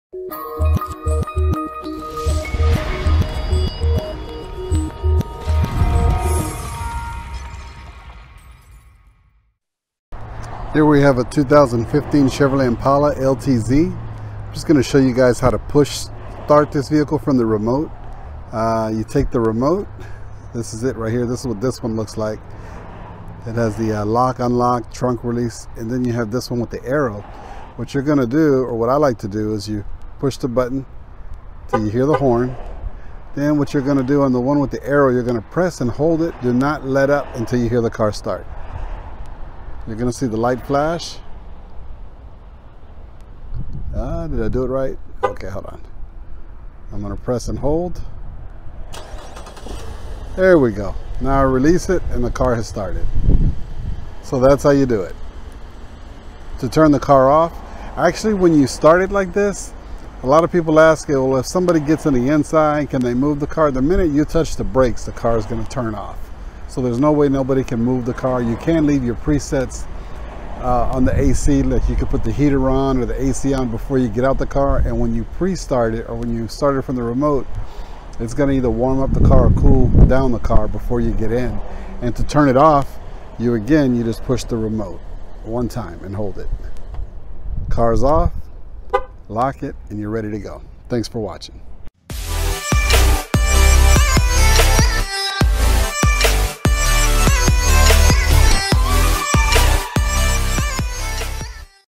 How to Remote Start a Chevrolet Impala
Quick video to show you how to remote start a 2014 to 2018 Chevrolet Impala with the keyless Remote.